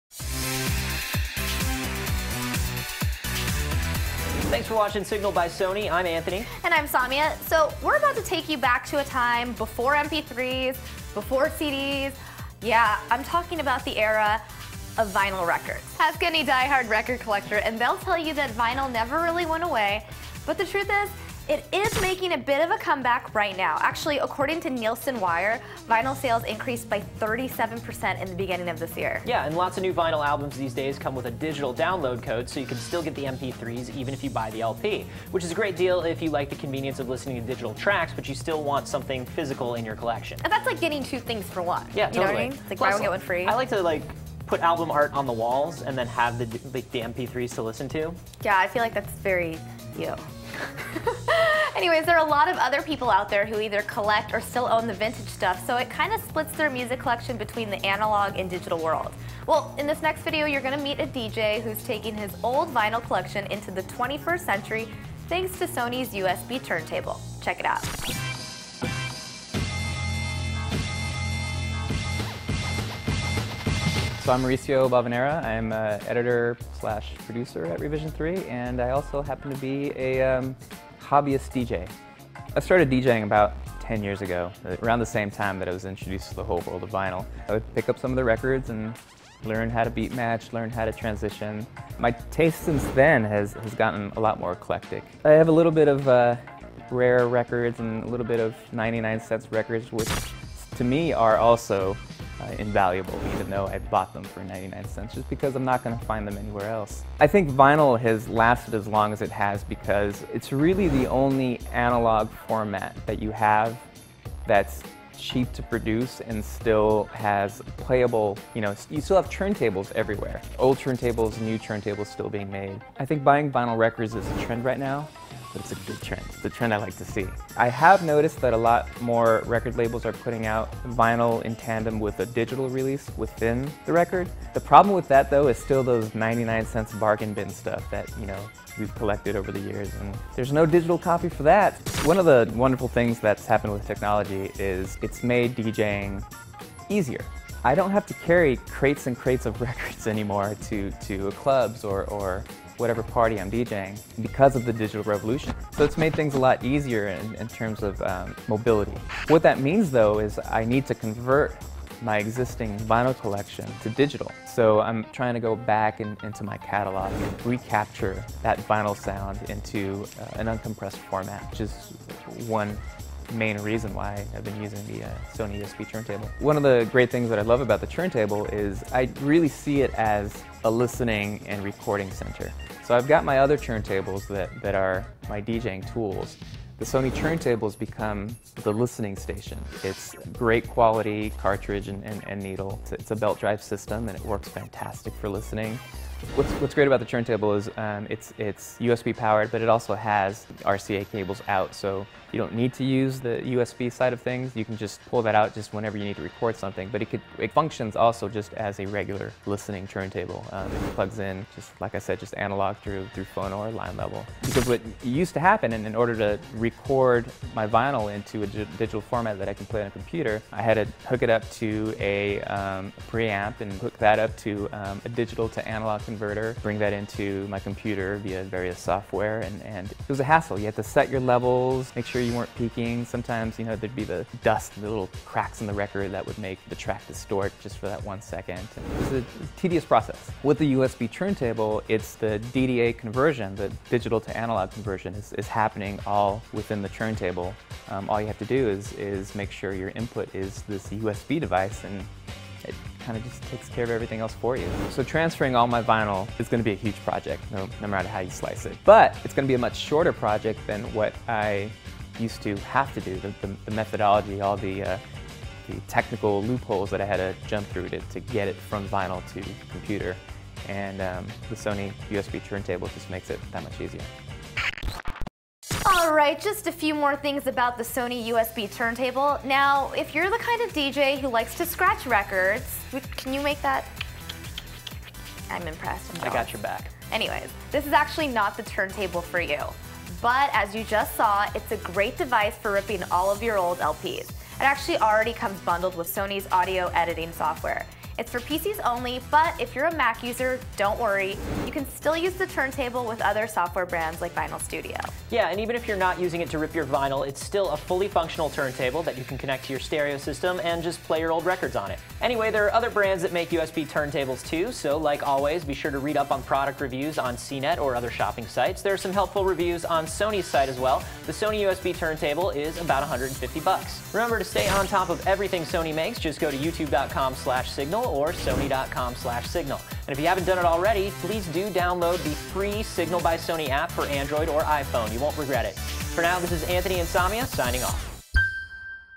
Analog Goes Digital with Sony's USB Turntable
Collecting vinyl is still a popular pastime after all these years, but even die-hard record collectors like the convenience of listening to mp3s or other digital audio formats. In this video, Team SGNL hears from a DJ hobbyist and vinyl enthusiast who's digitizing his entire record library using Sony's USB Turntable.